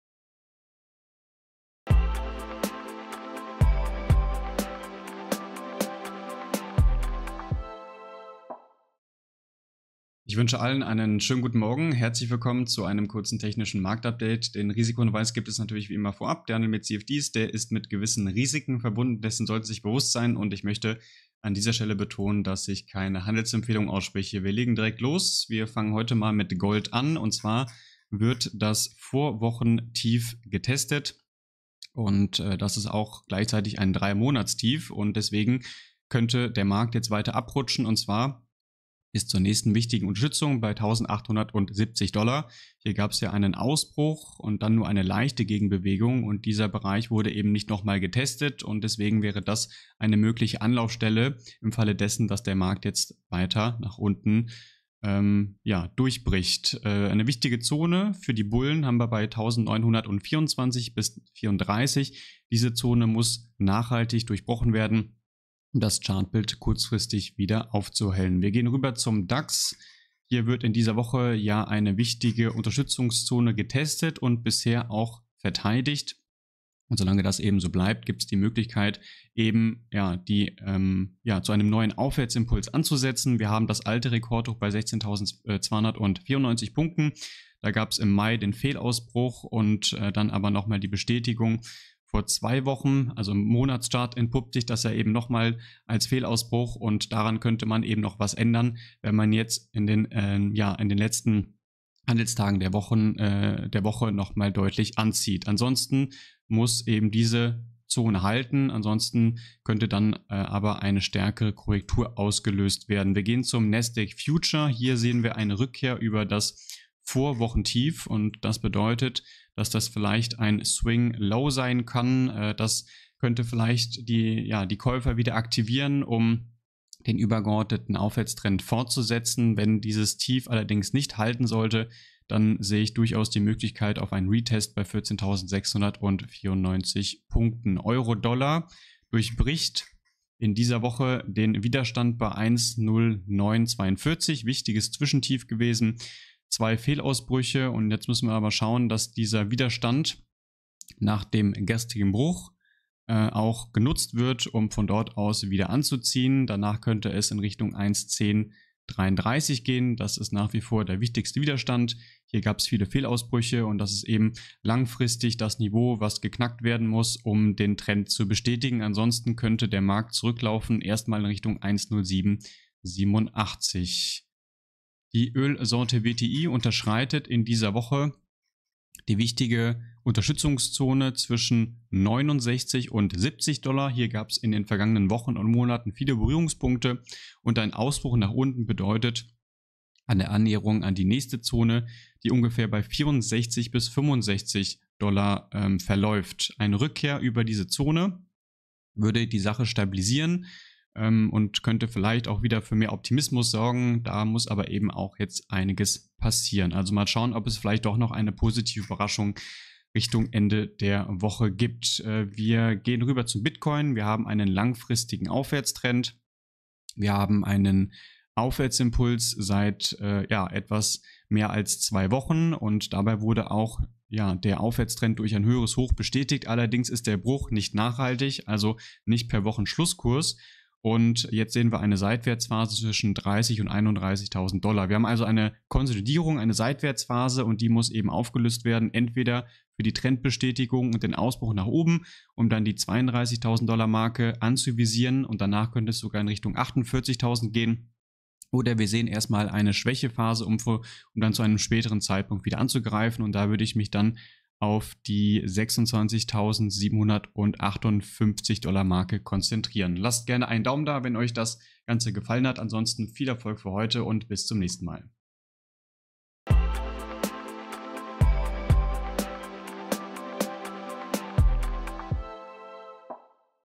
Gold unter Druck
Max Wienke von XTB Deutschland bespricht die Abwärtstendenz bei Gold. Dazu bespricht er wichtige charttechnische Marken. Auch besprochen werden Dax, Euro, Öl und Bitcoin.